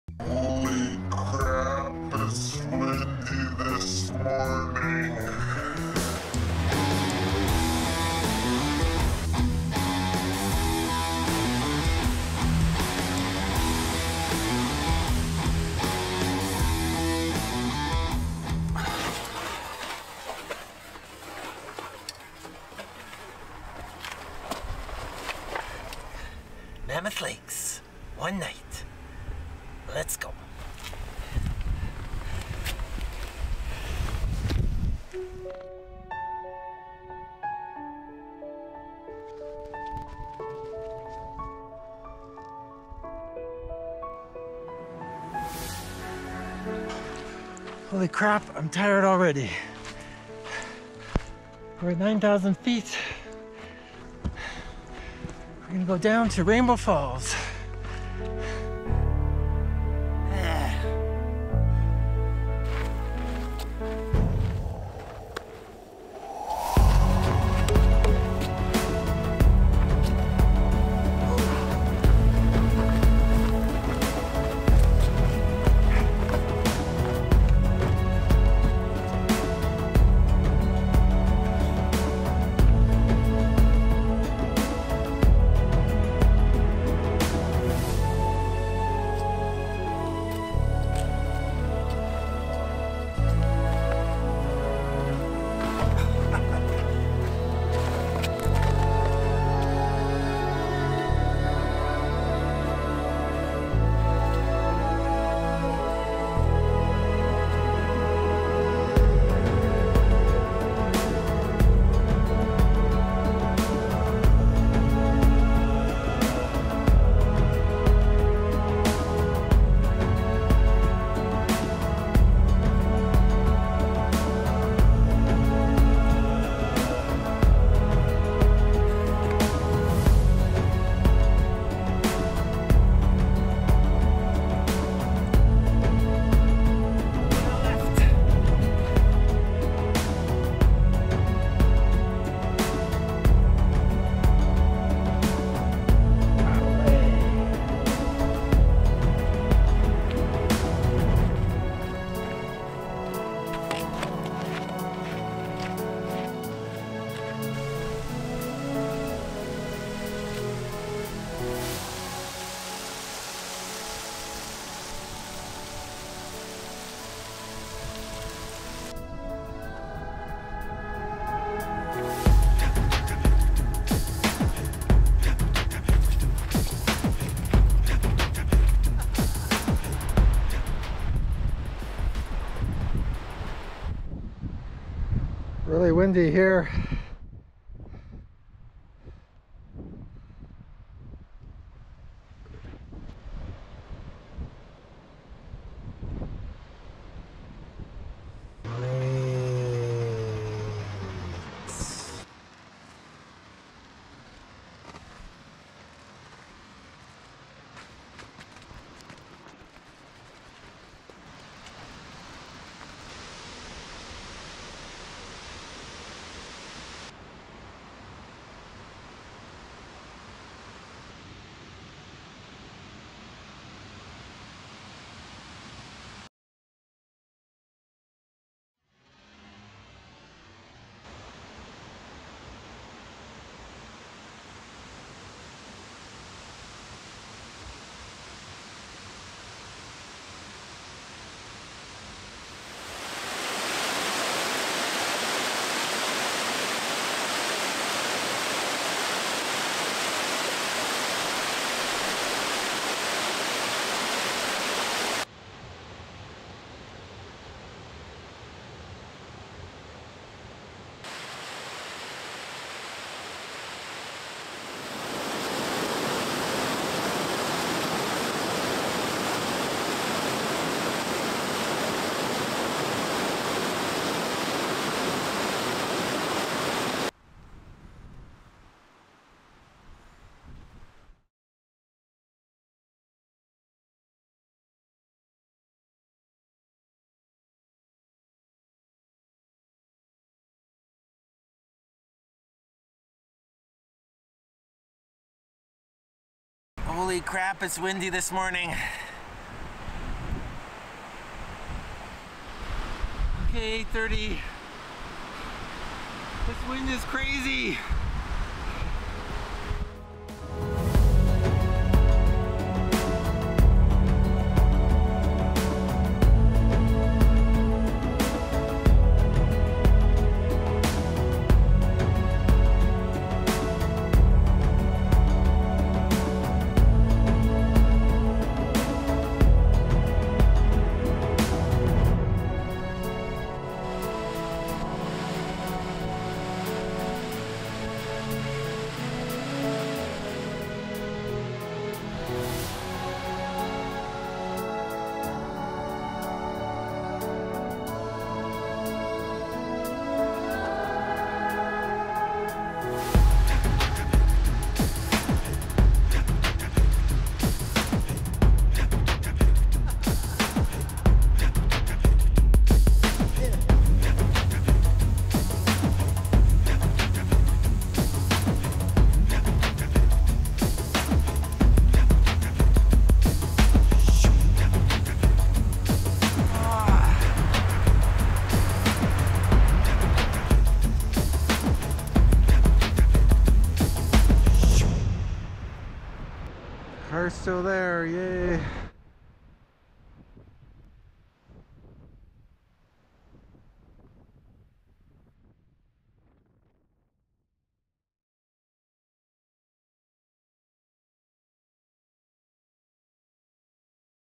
Backpacking Ansel Adams Wilderness | RAINBOW Falls @ Devils Postpile | Eastern Sierra waterfalls
Oct 2022. This is a one night backpacking adventure to beautiful Rainbow Falls in Devils Postpile National Monument, Ansel Adams Wilderness, at Mammoth Lakes in the Eastern Sierra Nevada, California.

It still had not snowed yet. The road to Devils Postpile National Monument is closed early due to construction.  There would be no one down at the waterfall. How about a quick overnight backpacking trip to Rainbow Falls? Heck yeah. Let’s do it! I was excited to go backpacking this weekend. Rainbow Falls is a pretty waterfall that flows year-round and with nothing much else happening in California I figured this should be a very good trip. Maybe I would see some Fall colors as well.

Waterfall Madman Adventures -